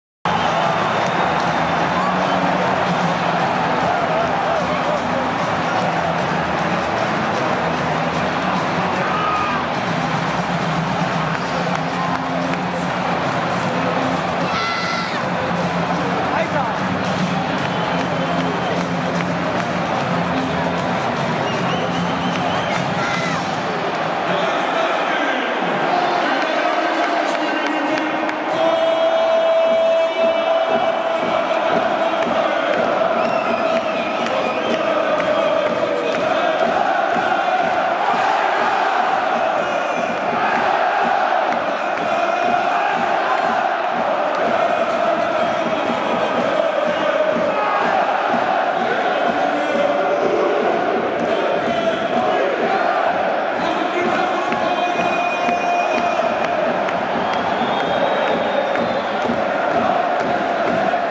Hannover 96 - Bayer 04 Leverkusen | Torjubel zum 2:2
Bundesliga Saison 2018 / 2019
10. März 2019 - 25. Spieltag
Hannover 96 - Bayer Leverkusen
Highlights von dem Spiel H96 & B04 in UHD.